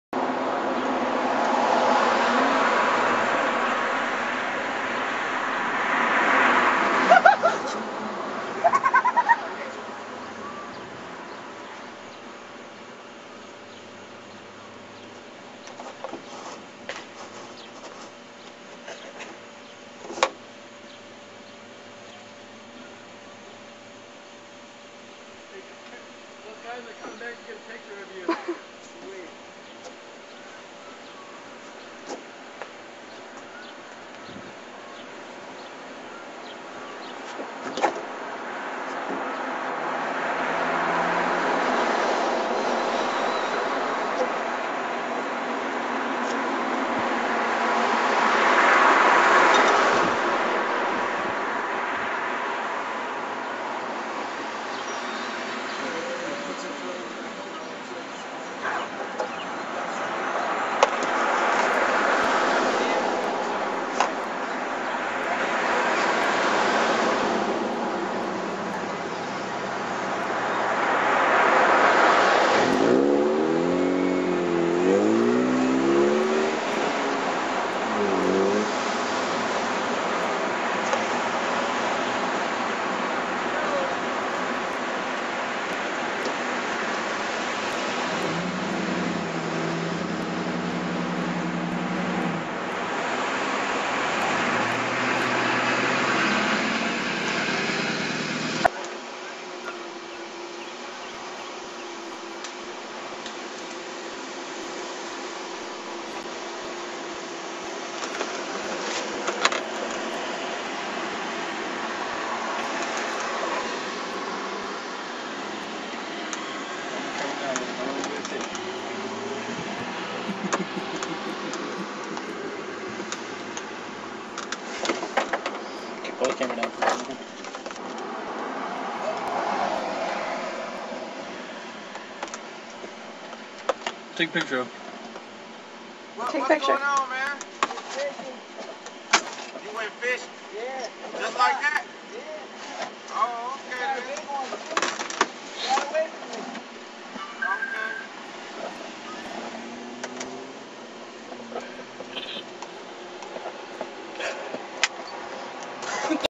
The Curtman Fishing on the sidewalk
The Curtman fishes on Huff Drive in Jacksonville, NC